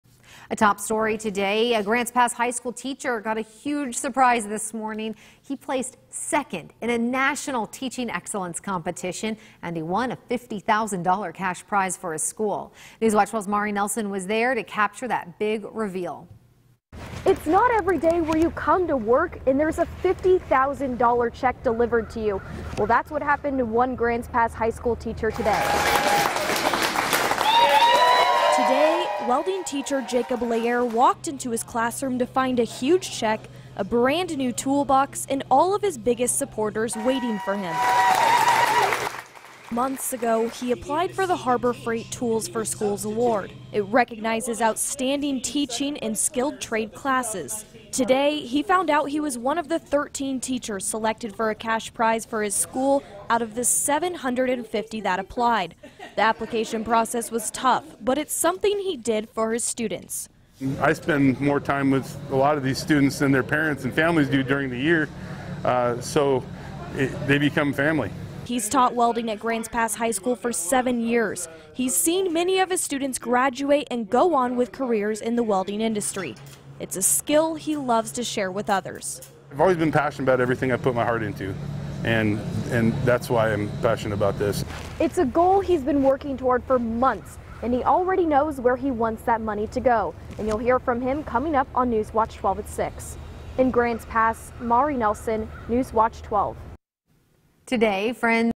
Grants Pass teacher wins $50K for school in national competition
Welding teacher Jacob Leair received the prize from Harbor Freight 'Tools for Schools.'